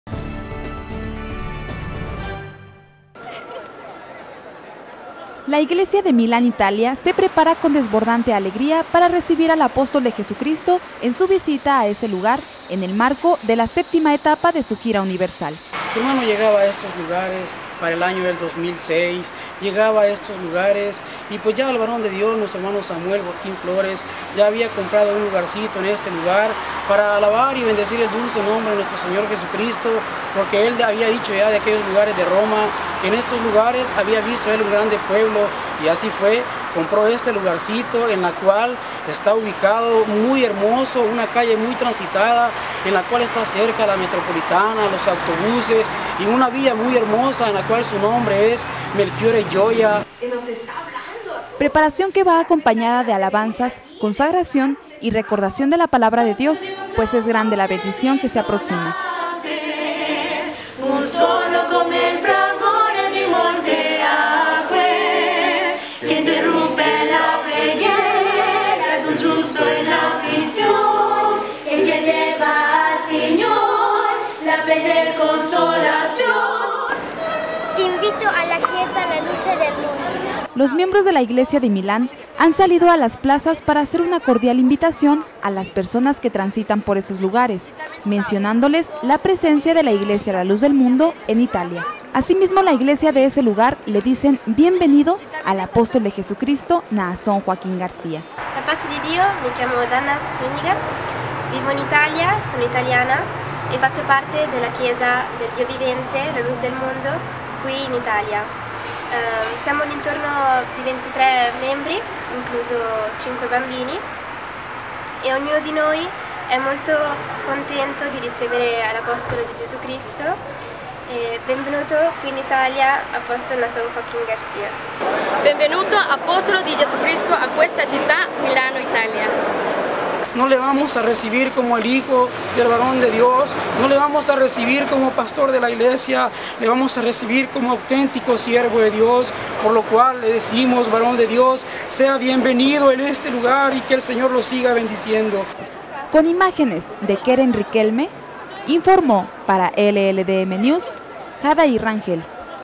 Milán Italia se prepara para recibir al Apóstol de Jesucristo
Noticias más relevantes de la Iglesia la Luz del Mundo.